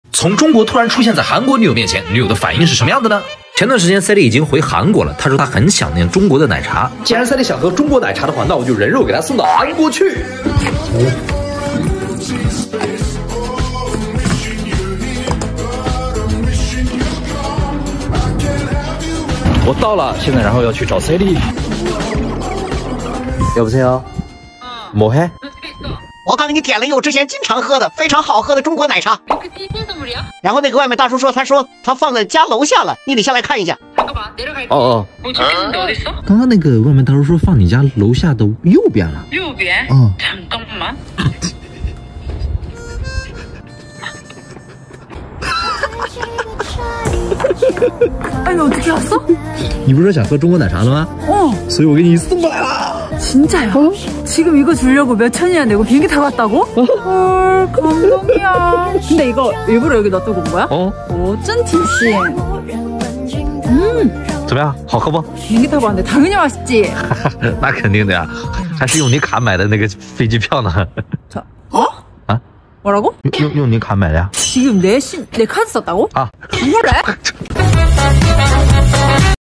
高甜时刻！给韩国女友人肉送奶茶，惊喜变惊吓？韩国
我是钟吾澡雪，纪录我们的爱情故事。
🇨🇳男生和🇰🇷女生
甜甜的爱情开始咯💖💖💖💖💖💖💖💖
中韩情侣的日常🤣🤣